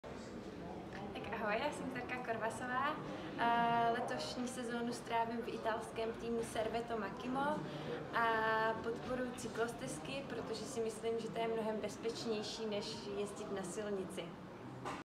Tereza KORVASOVÁ
Jsem členka reprezentačního týmu ČR a Italského UCI týmu Servetto Makhymo Beltrami TSA

Dvojnásobná mistryně republiky v časovce jednotlivců Tereza Korvasová na kole na rozdíl od mnohých nevyrůstala. Rodačka ze slováckých Vnorov se nejprve po vzoru svého tatínka věnovala atletice v tamním oddíle AK Hodonín. O tom , že Tereza byla vytrvalecký talent svědčí mimo jiné i 13 mistrovských titulů ze středních tratí a 4. místo z MS juniorů v běhu do vrchu. Její sportovní cesta ale nakonec vede jinou cestou. Pak přesedlala na kolo a začínala s cyklistikou v týmu Sokol Louny, když pak oblékla dres armádního týmu Dukla Praha, kde se prosadila i v tomto sportu do České reprezentace.